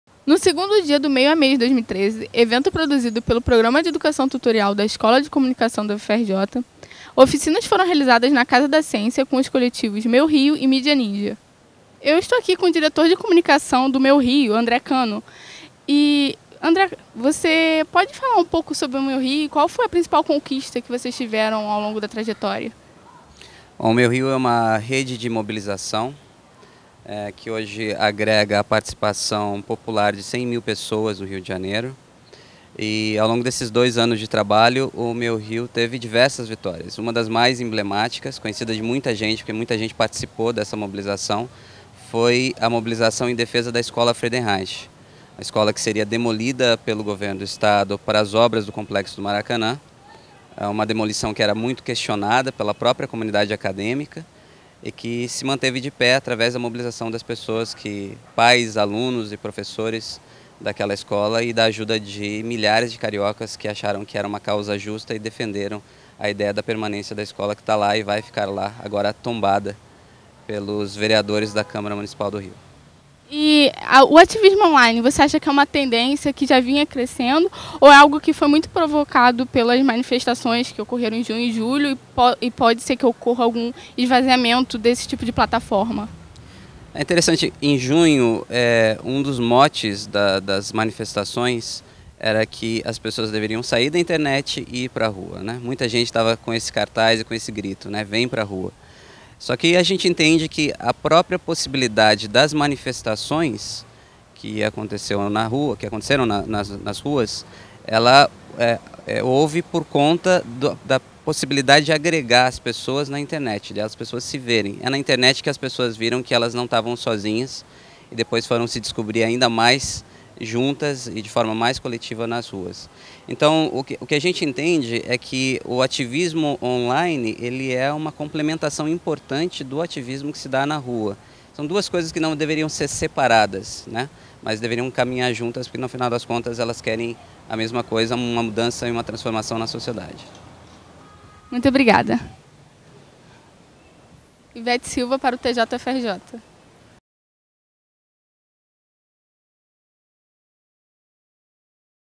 Mídia-Ativismo é o tema no segundo dia do Meio a Meios 2013
TJ UFRJ conversou nesta quinta-feira, 28 de novembro de 2013, com André Kano, diretor de comunicação do movimento Meu Rio.